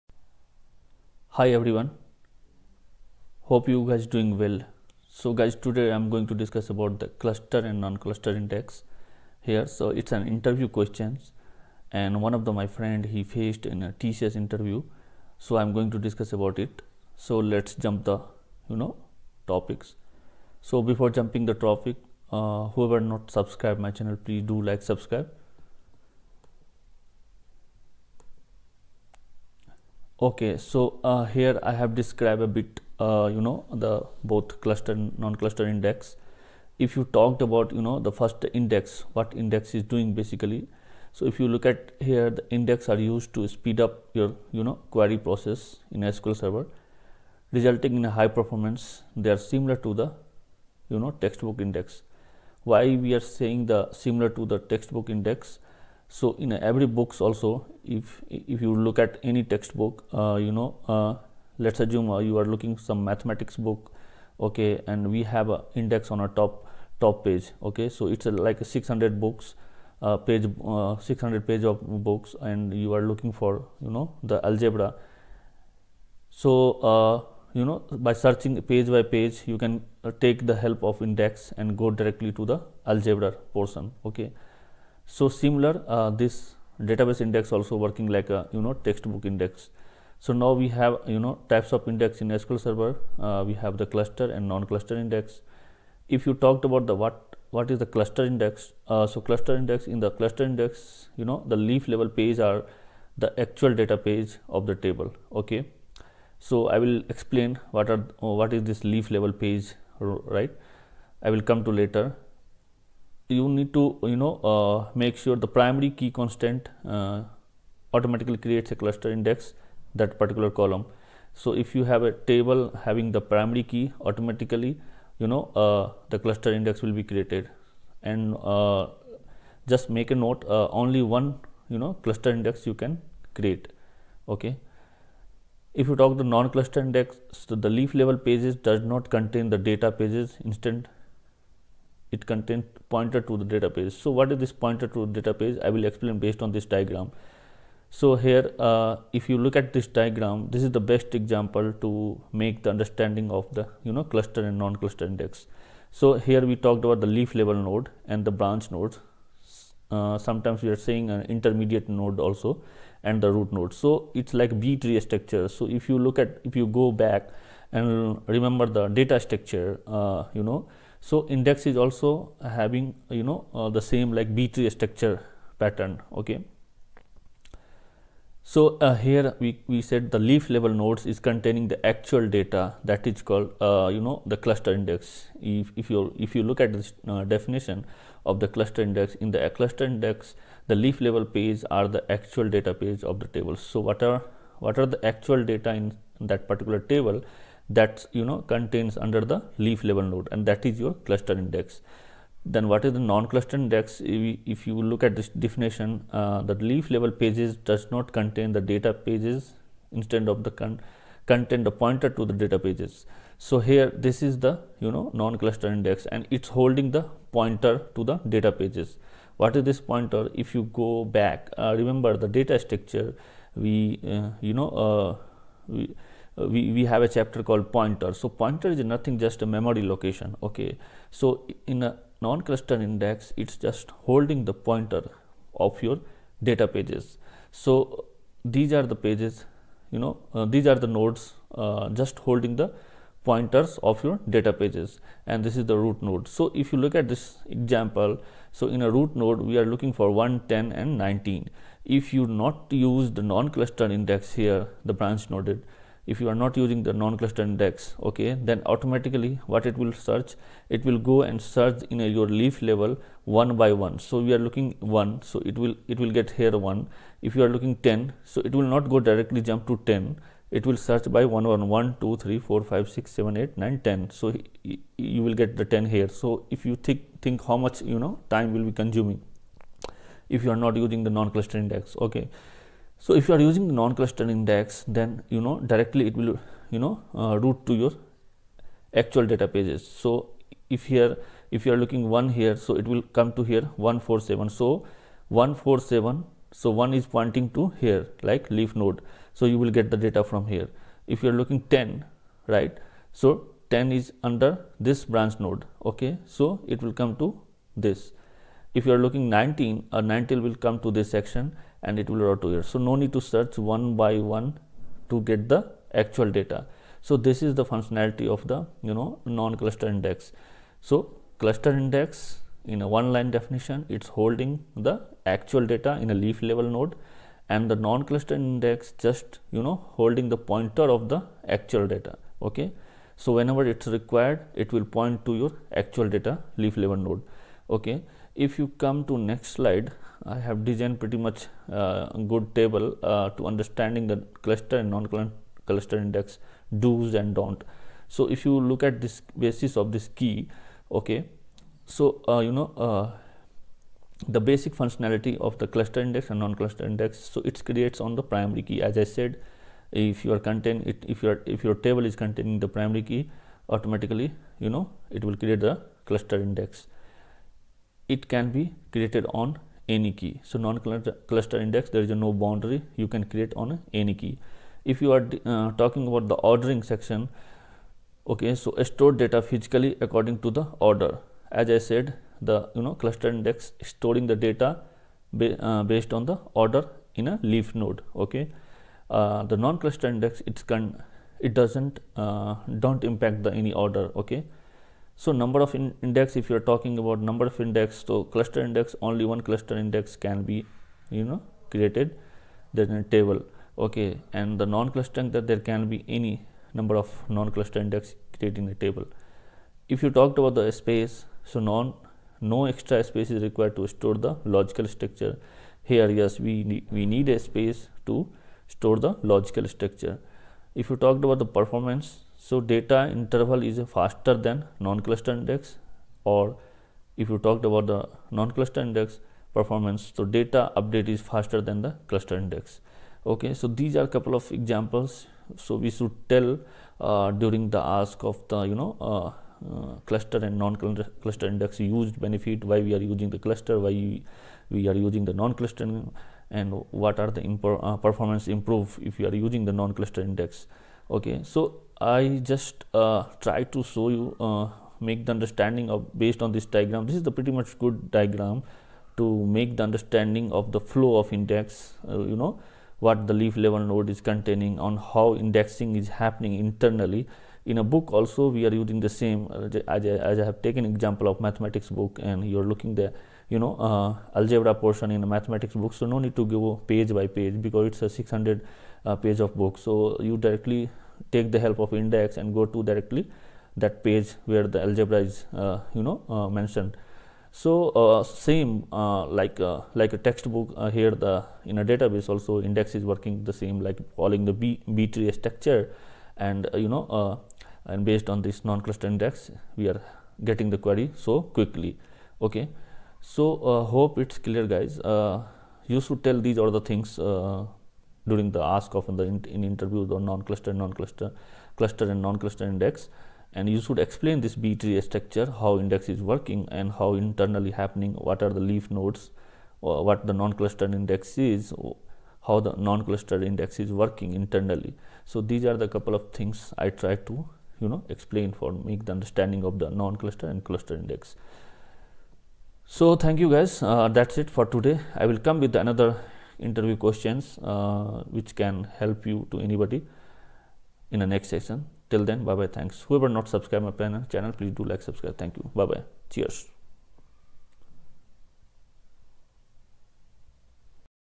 TCS Interview Question Clustered /Non -Clustered Index, How Index is working
Hi Mate,

In this session we are going to discuss about how index is working ? what is clustered/ non clustered index, how index improves performance,  type of index, interview questions of indexing.